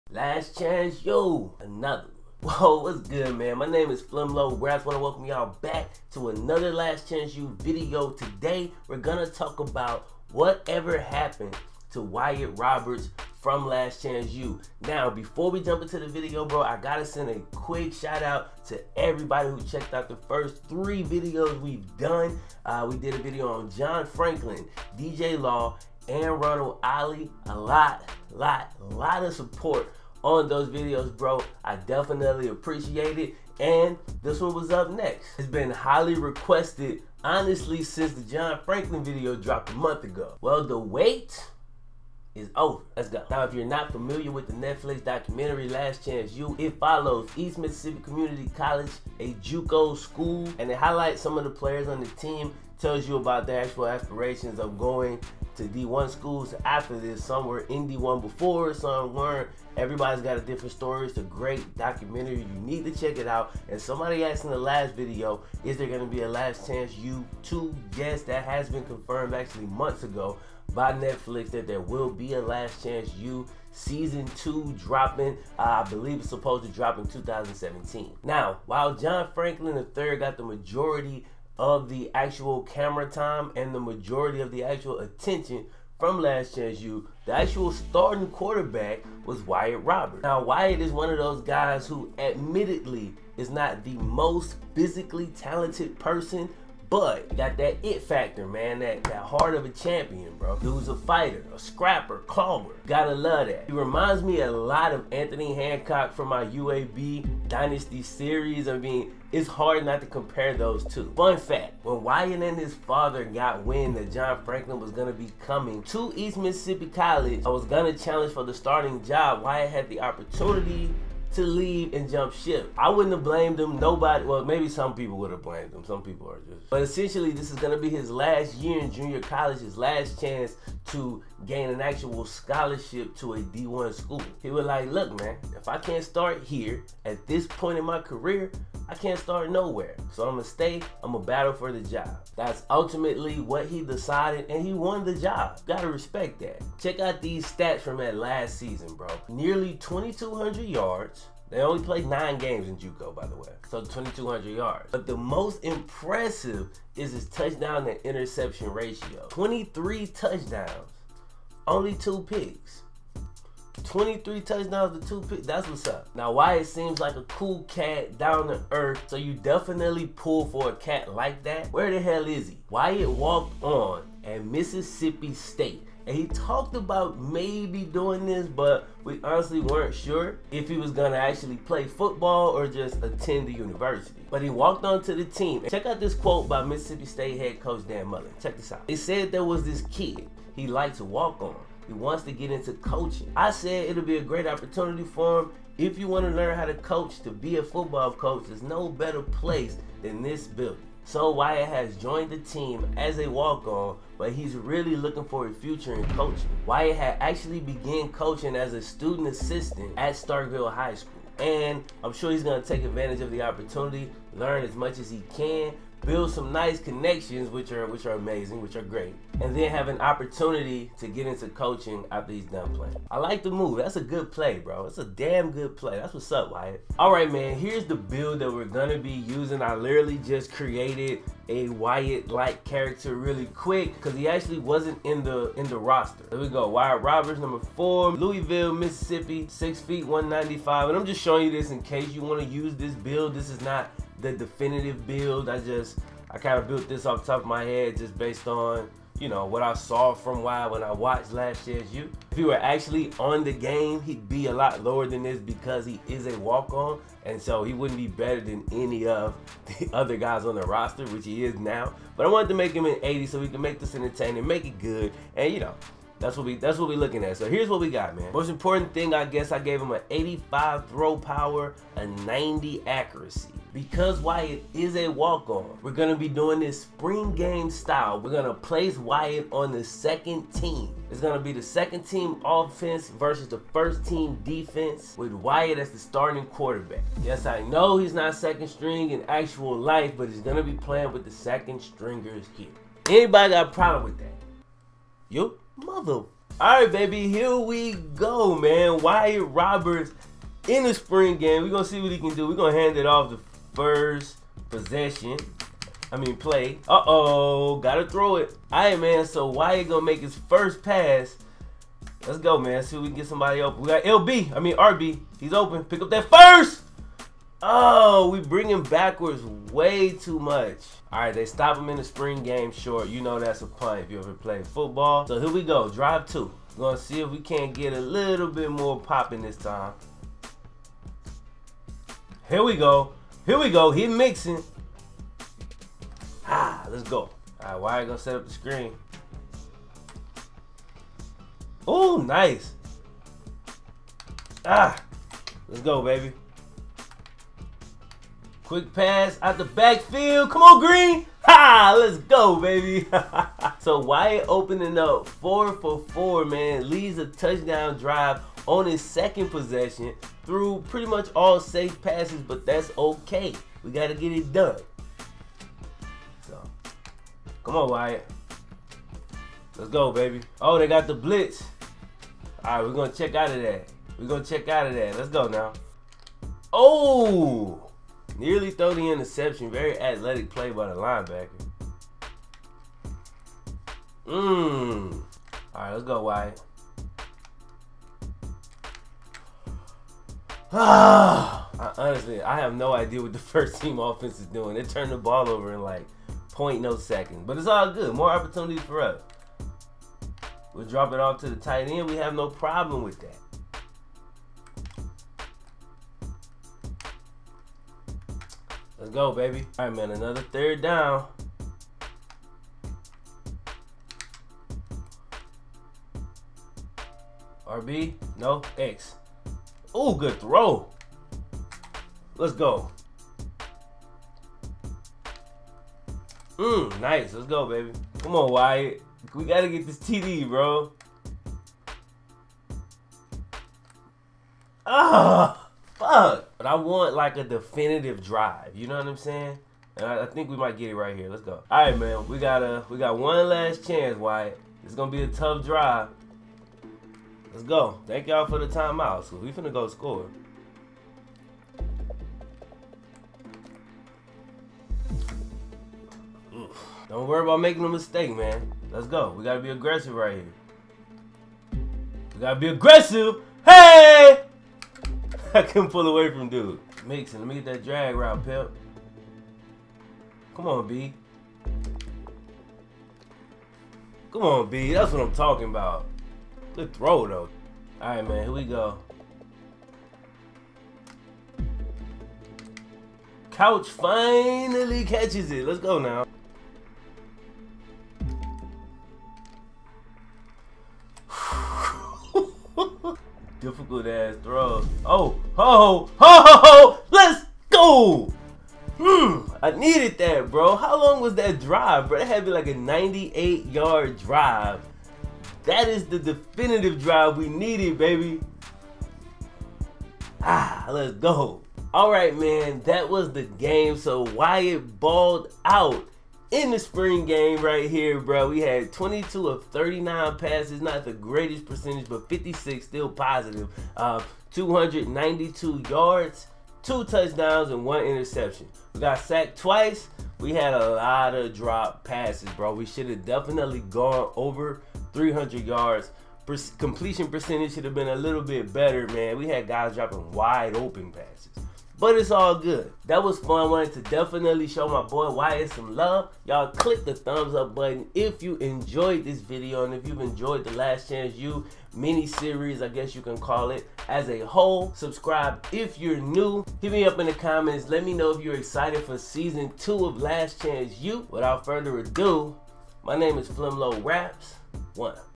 WHAT EVER HAPPENED TO WYATT ROBERTS FROM LAST CHANCE U???
wyatt roberts was the strating qb on the netflix documentary last chance u. although john franklin got most of the attention...wyatt was a steady qb who has now beem granted an opportunity to furthur his career at Mississippi state